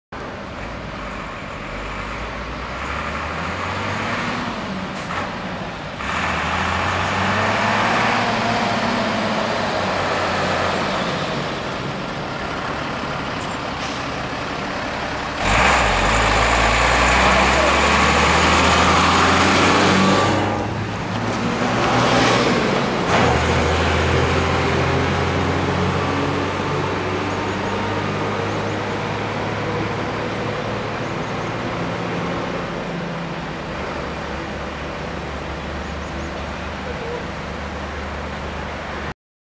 RW 1 FF Hagen LG 21 Boele-Kabel
Der Rüstwagen 1 (RW 1 / 21/51-1) der Freiwilligen Feuerwehr Hagen, Löschgruppe 21 Boele-Kabel auf Verlegungsfahrt.
Copyright : ZieglerHLF